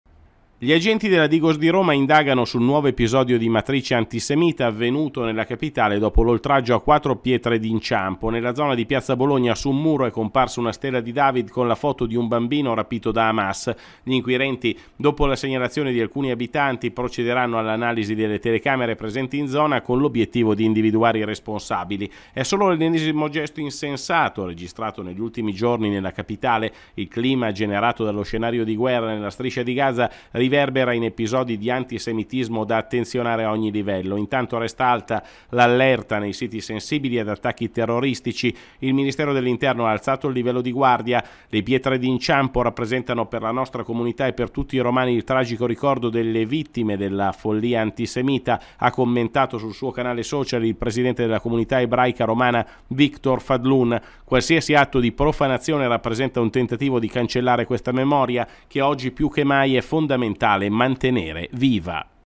Pietre d'Inciampo imbrattate, indaga la DIGOS
Gli agenti della Digos di Roma indagano su un nuovo episodio di matrice antisemita avvenuto nella Capitale dopo l'oltraggio a 4 pietre d'inciampo. Nella zona di piazza Bologna, su un muro, è comparsa una stella di David con la foto di un bambino rapito da Hamas. Gli inquirenti, dopo la segnalazione di alcuni abitanti, procederanno all'analisi delle telecamere presenti in zona con l'obiettivo di individuare i responsabili. E' solo l'ennesimo gesto insensato registrato negli ultimi giorni nella capitale. Il clima generato dallo scenario di guerra nella Striscia di Gaza riverbera in episodi di antisemitismo da attenzionare a ogni livello. Intanto resta alta l'allerta nei siti sensibili ad attacchi terroristici. Il Ministero dell'Interno ha alzato il livello di guardia. “Le pietre d’inciampo rappresentano per la nostra Comunità e per tutti i romani il tragico ricordo delle vittime della follia antisemita”, ha commentato sul suo canale social il presidente della Comunità Ebraica Romana, Victor Fadlun. “Qualsiasi atto di profanazione rappresenta un tentativo di cancellare questa memoria, che oggi più che mai è fondamentale mantenere viva” ha concluso Fadlun.

DOVE SEGUIRCI